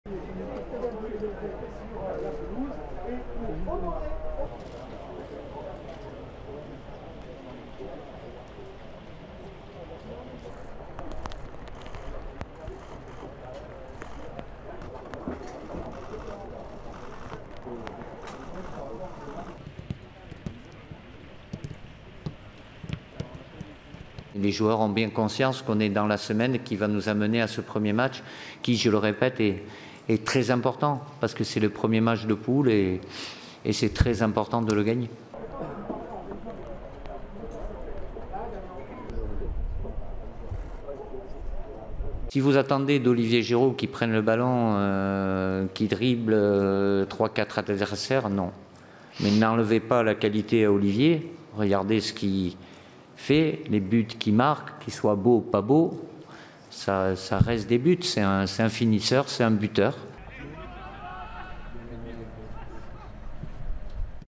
Mondial-2018: la France s'entraîne pour la 1re fois à Istra
L'équipe de France fait son premier entraînement en Russie, au lendemain de son arrivée: l'aventure de la Coupe du monde 2018 est lancée, avec l'Australie samedi à l'horizon.